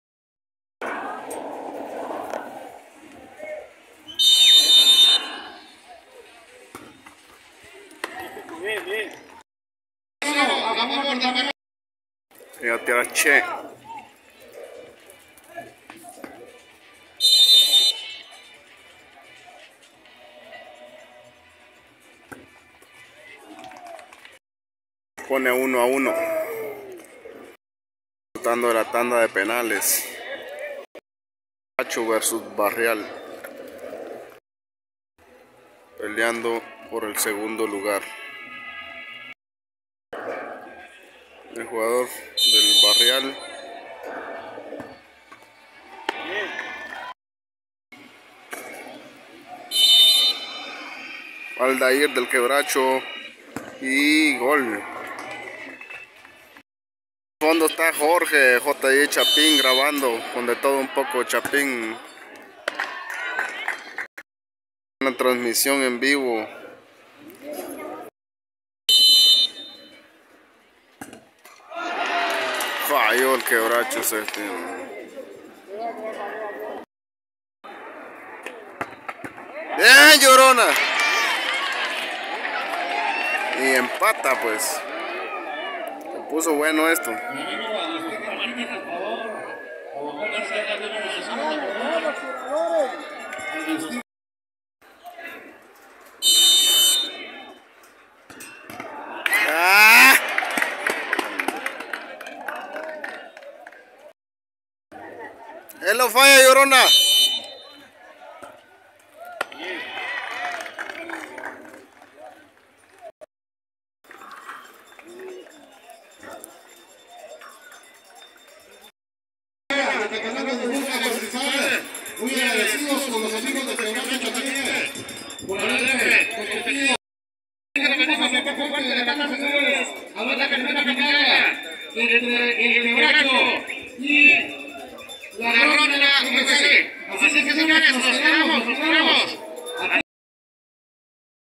Así se vivieron los penales por el 3er lugar FC Quebracho vs Barreal
Este video fue grabado por JY chapin de un ángulo más cercano para que pudieran disfrutar de los penales que vivieran el ambiente y los muchachos del fc Quebracho disputaron quedando en 4to lugar y estamos felices porque dieron un espectáculo de fútbol apesar de no tener campo el Quebracho salió con un 4to y 1er lugar bendiciones para ti que ves los videos

Los derechos de este video pertenecen a JY Chapin ®